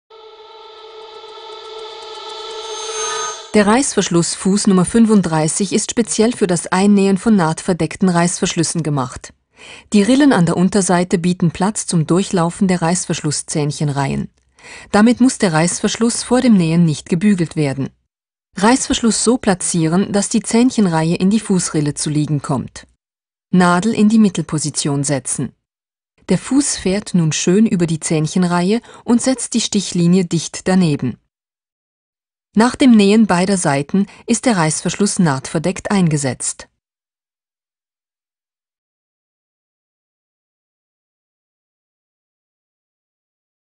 30) BERNINA Nähfüsse - Reissverschlussfuss Nr. 35
In der 30. Lektion des BERNINA Nähfuss Online-Tutorials zeigen wir Ihnen die Möglichkeiten des Nähfusses für nahtverdeckte Reissverschlüsse Nr. 35. Mit diesem Nähfuss können Reissverschlüsse verdeckt eingenäht werden. Das Einnähen von nahtverdeckten Reissverschlüssen ist mit diesem Nähfuss sehr einfach, da die Rillen an der Unterseite des Nähfusses Platz zum Durchlaufen der Reissverschlusszähnchenreihen bieten. Der Reissverschluss muss daher vor dem Nähen nicht gebügelt werden. Nahtverdeckte Reissverschlüsse bieten die elegante Möglichkeit, Schlitze an Kleidungsstücken unauffälig zu schliessen und finden auch bei Heimdekorationen und Accessoires Anwendung. Schauen Sie sich auch die weiteren Lektionen des Nähfuss-Tutorials an und erfahren Sie mehr über BERNINA Nähmaschinen